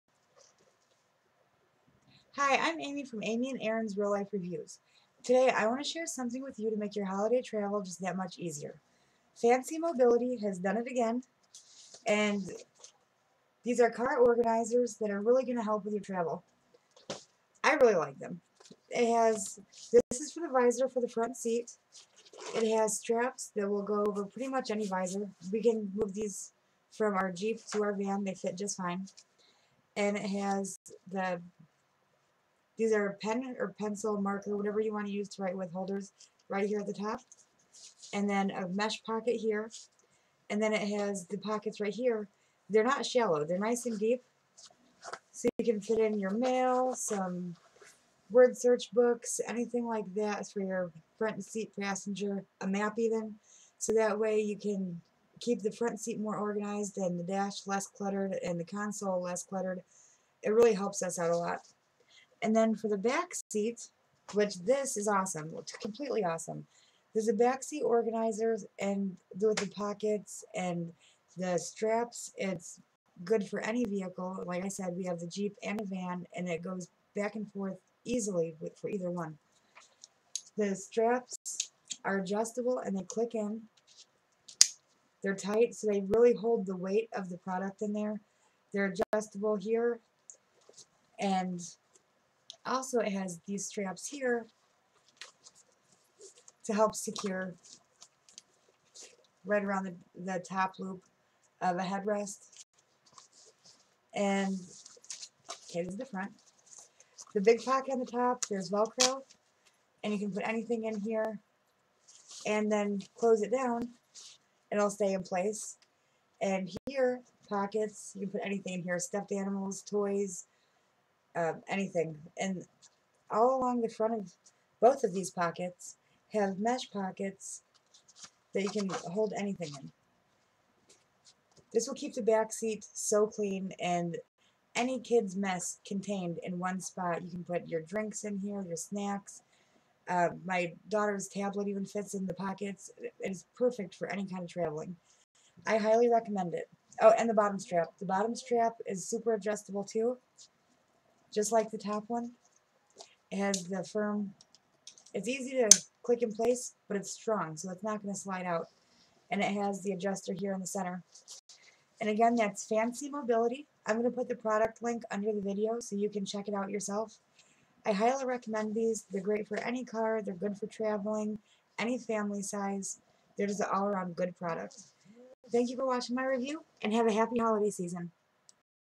FancyMobility Review Backseat Car Organizer - Kids Toy Storage - Comes with Visor Organizer
One of the things that make me feel safer is not having stuff in the floor. Our family is messy when it comes to traveling. Fancy Mobility has made a difference in our travel. They have a combo pack of a back seat organizer and a visor organizer.

Have you seen the vehicles that have 6 weeks of mail in the front and a toy box in the back? Well, take a trip to my driveway, you will see two.

That's why I love these organizers so much. I will start with the visor. It has tight elastic bandy that fit securely to the visor. It has 2 long/deep pockets that is perfect for mail. I like to keep my mail in the one in the back. Aron likes to keep his crossword puzzles in the one in the front. The short pouch can hold any small thing and it even has 2 places for pens.

The back seat organizer is perfect for all of the girls play stuff. The top pouch has a velcro closure. It has 2 rows of pockets that also have mesh over pockets. This can hold just about anything she can throw at it.

These make our trips safer and easier. When you have a little girl in the back seat, mom, where is..., mom I can't reach..., mom do you have  pen in your purse?, mom can I play on your phone?, mom, mom, mom. It is very distracting.

When she has all of her stuff right there, in reach, that she can get for herself, it is so much more peaceful of a ride.